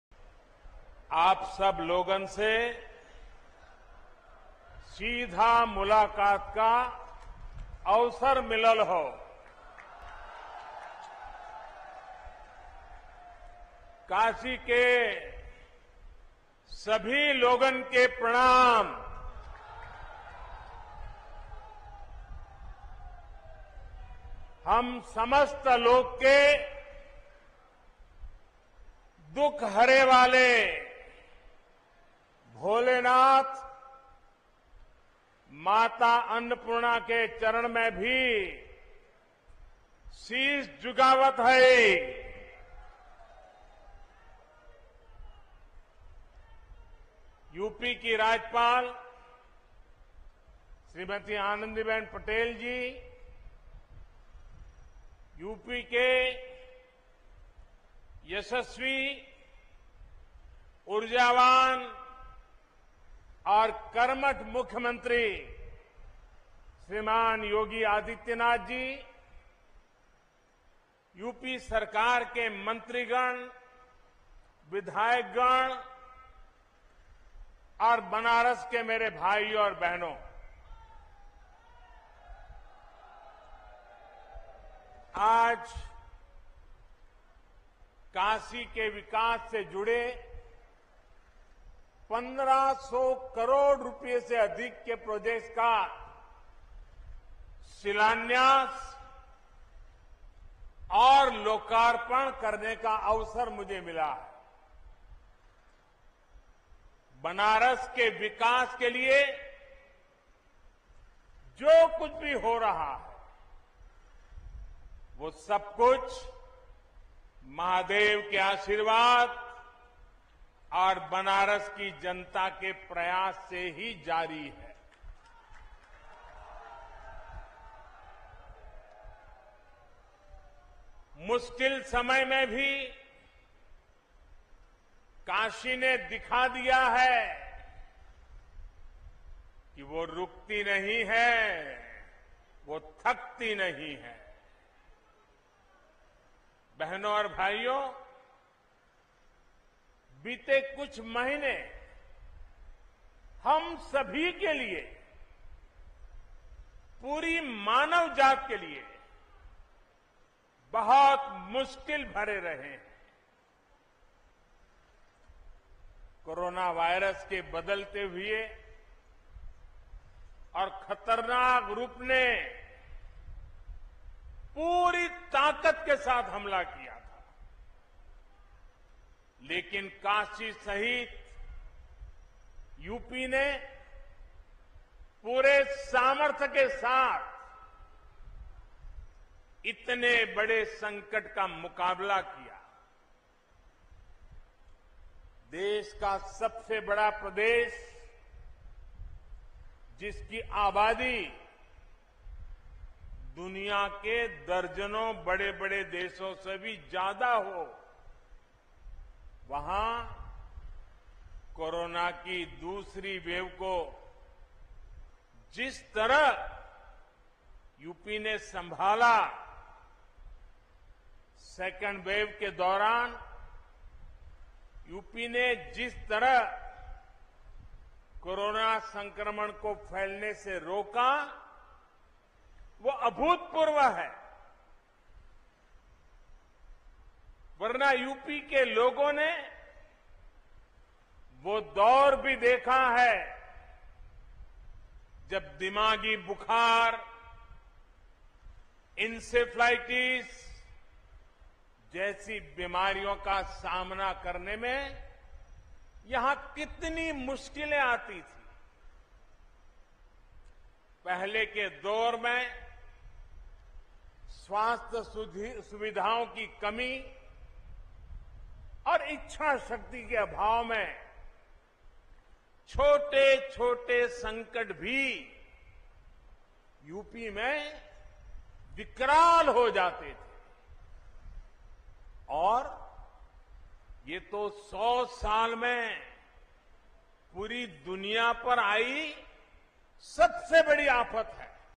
PM Modi का सामने आया भोजपुरिया अंदाज, बोले- काशी के सभी लोगन के प्रणाम |Assam Headlines News.
PM Modi का सामने आया भोजपुरिया अंदाज, बोले- काशी के सभी लोगन के प्रणाम